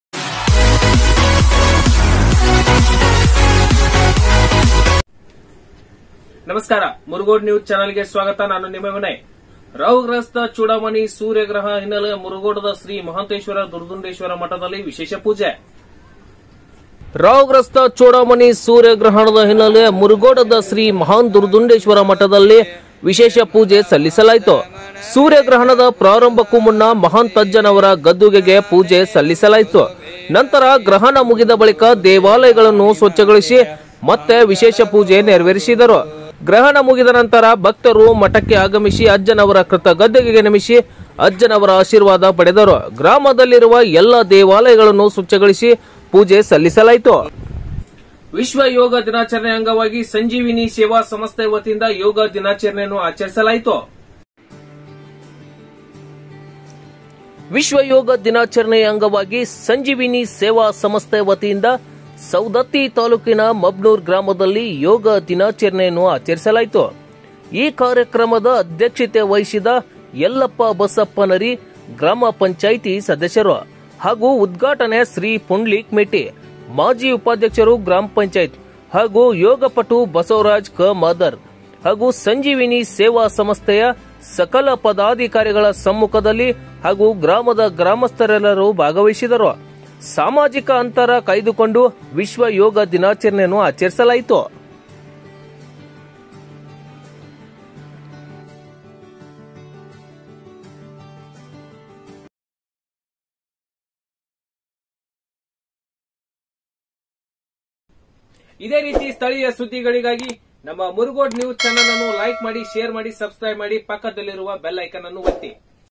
# murgod news#ಮುರಗೋಡ ನ್ಯೂಸ್ ಸವದತ್ತಿ ತಾಲೂಕಿನ ಮಬನೂರ ಗ್ರಾಮದಲ್ಲಿ ಅಂತರಾಷ್ಟ್ರೀಯ ಯೋಗ ದಿನಾಚರಣೆ ಆಚರಿಸಲಾಯಿತು
ರಾಹು ಗ್ರಹಸ್ತ ಚೂಡಾಮಣಿ ಸೂರ್ಯಗ್ರಹಣದ ಹಿನ್ನೆಲೆಯಲ್ಲಿ ಮುರಗೋಡದ ಮಹಾಂತ ದುರ್ದುಂಡೇಶ್ವರ ಮಠದಲ್ಲಿ ವಿಶೇಷ ಪೂಜೆ